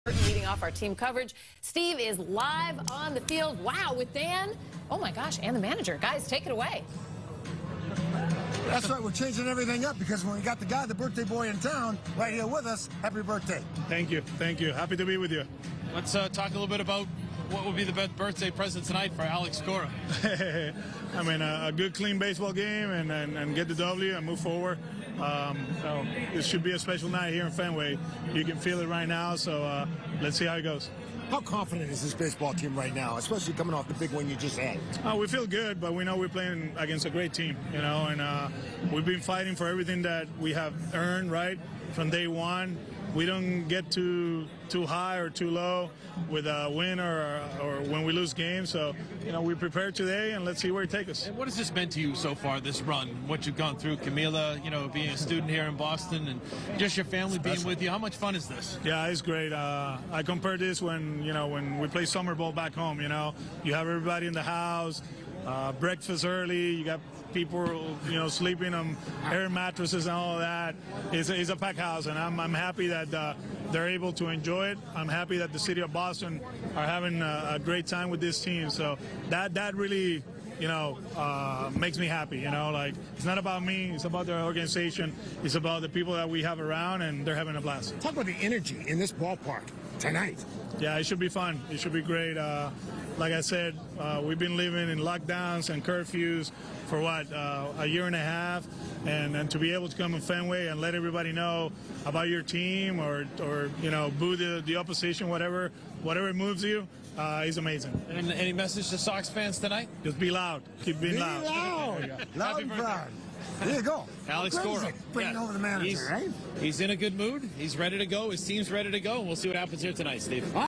Alex Cora: 'It Should Be A Special Night At Fenway'
WBZ-TV's Dan Roche and Steve Burton spoke with Red Sox Manager Alex Cora ahead of ALCS Game 3 at Fenway Park.